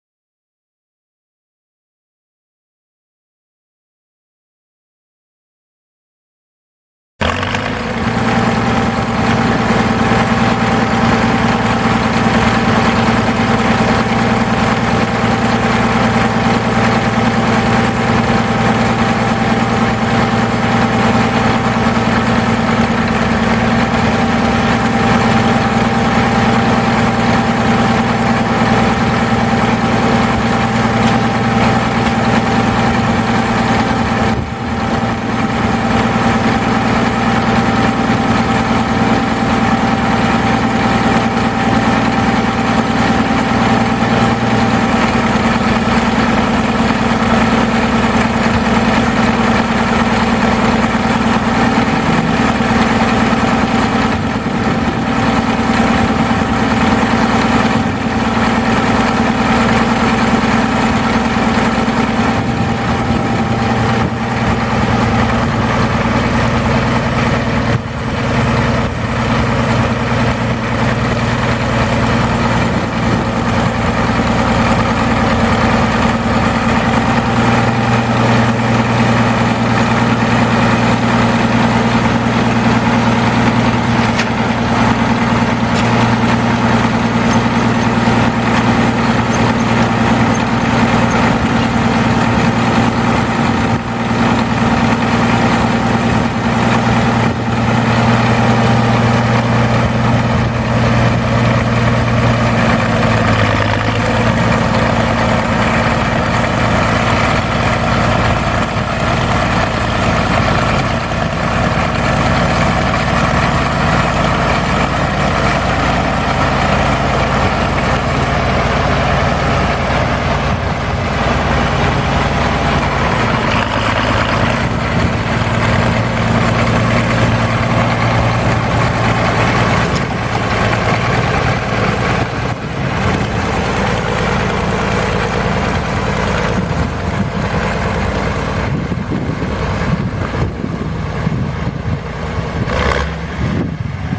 1964 Caterpillar 12E motor grader for sale | sold at auction December 29, 2011
1964 Caterpillar 12E motor grader
7,326 hours on meter
Caterpillar D333 six cylinder diesel engine
115 HP
Group# 2M4050
Manual transmission
6F-2R gears
Cab with heater
Bench seat
14&#039; moldboard
Scarifier
Scarifier control
14.00R24 tires
24,400 lbs. base operating weight
Serial 99E8866

It was located in Quinter, KS, and we sold it to the highest bidder for $9,900.